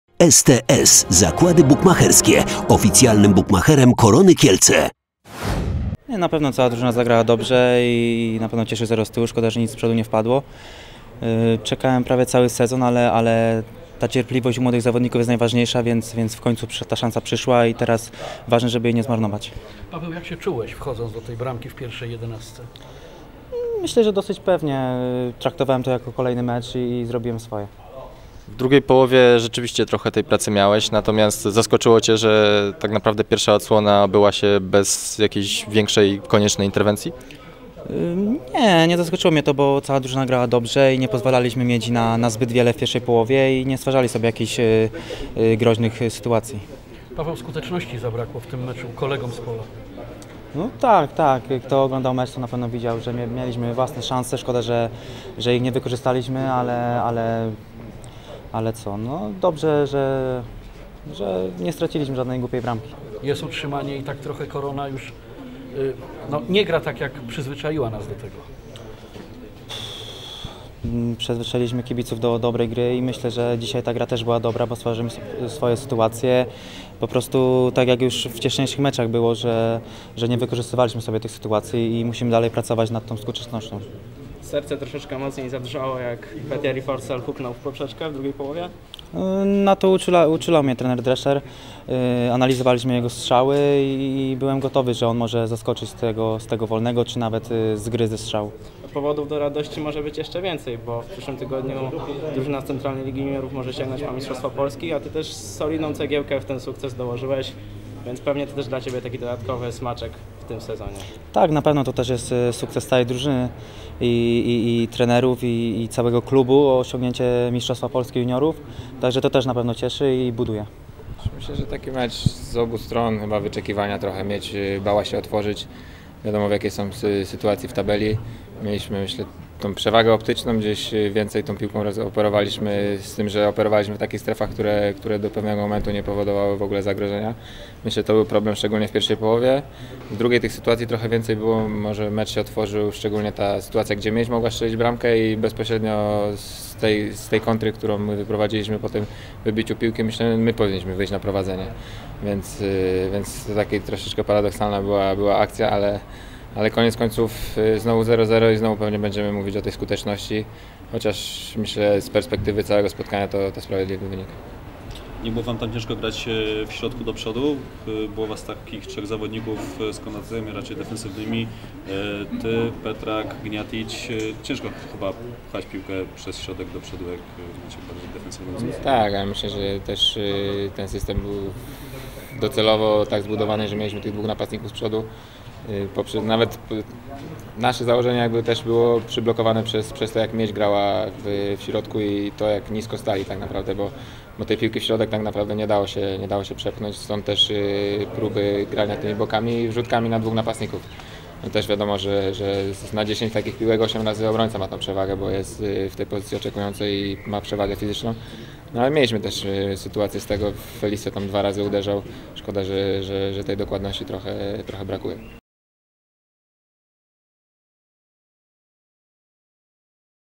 Sokół i Żubrowski po meczu Korona Kielce - Miedź Legnica 0:0 (04.05.2019 r.)
- Czekałem prawie cały sezon na tę szansę, cierpliwość u młodych zawodników jest najważniejsza, cieszę się, że okazja na grę w pierwszym składzie wreszcie przyszła. Na boisku czułem się dosyć pewnie, traktowałem to jako kolejny mecz i zrobiłem swoje - oceniał po debiucie w Ekstraklasie Paweł Sokół. Mecz z Miedzią podsumował również Jakub Żubrowski.